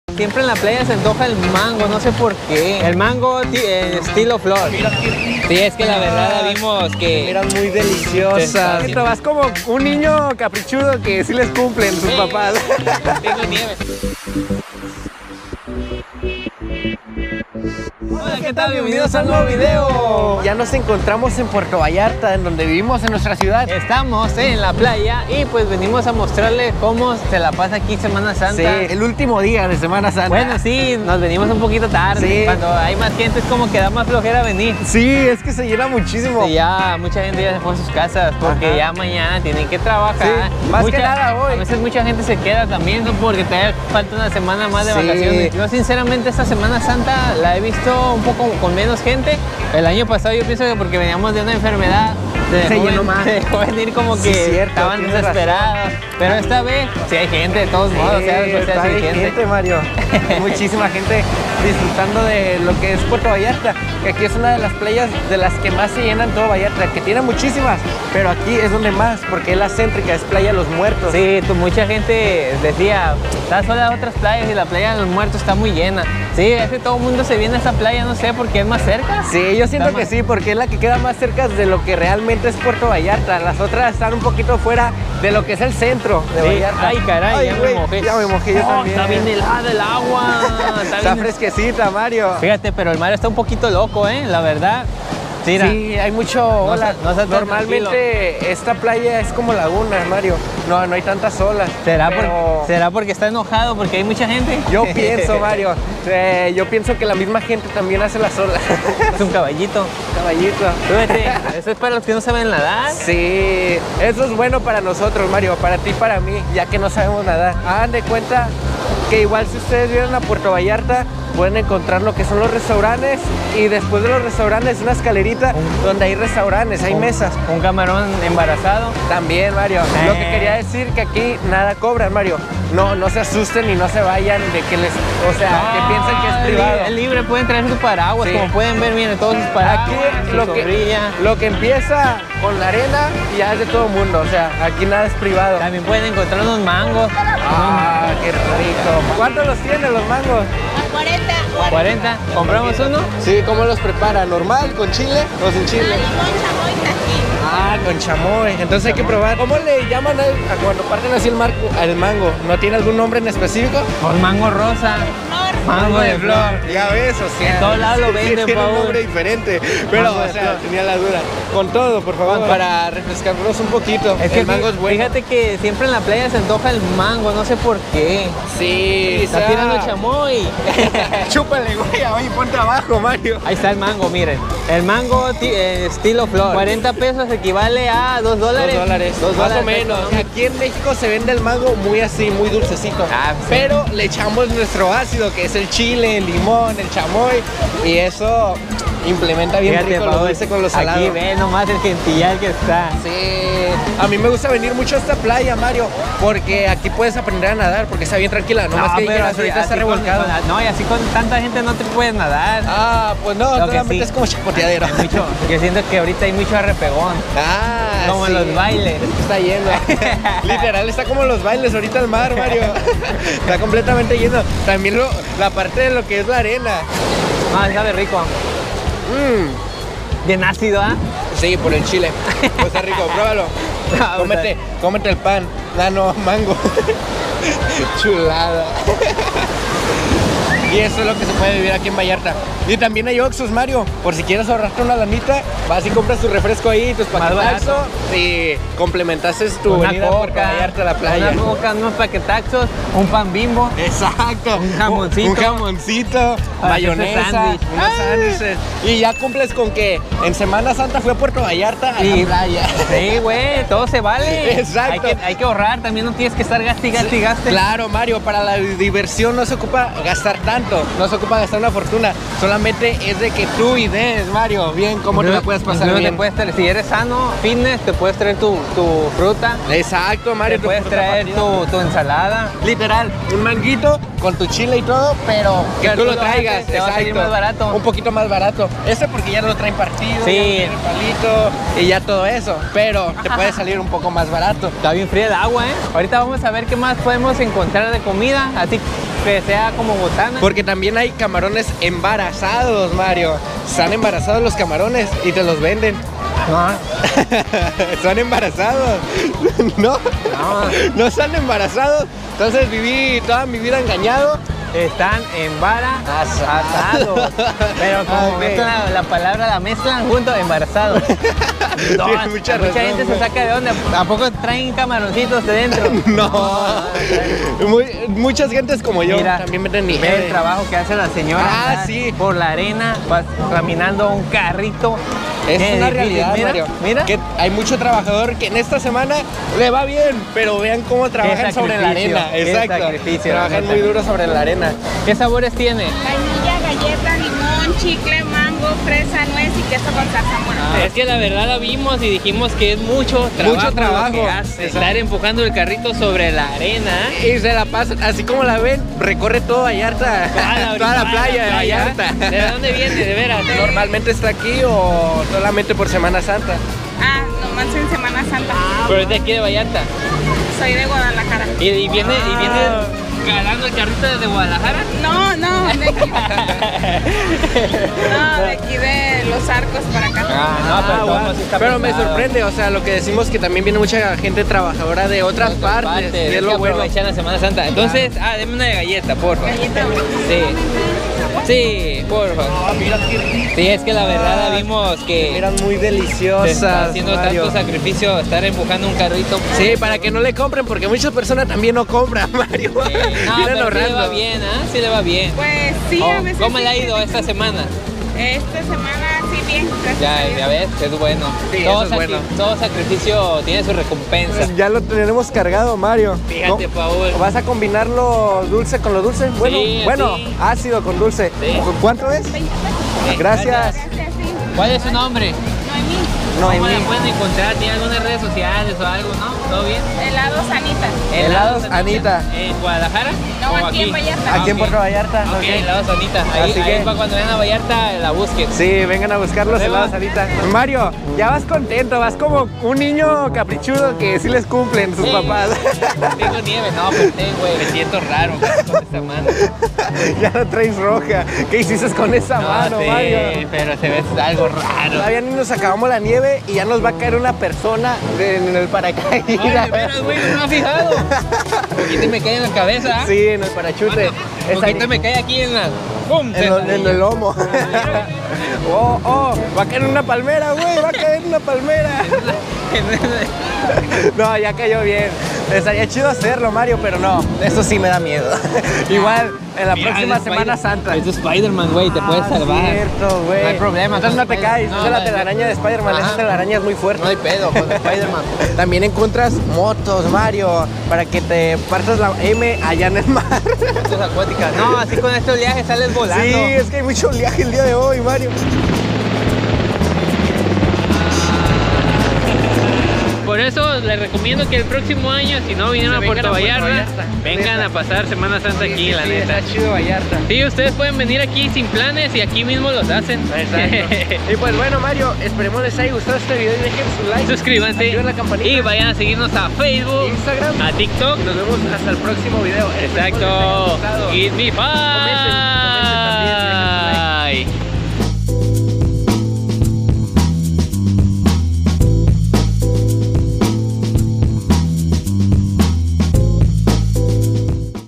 SEMANA SANTA EN MÉXICO 🌊🤯 *Puerto Vallarta* 🌴⛵ // Mario y Paul
Fuimos a caminar en Vacaciones de Semana Santa a la bonita Playa los Muertos ubicada en Puerto Vallarta México y fuimos haciendo paradas para comer y probar postres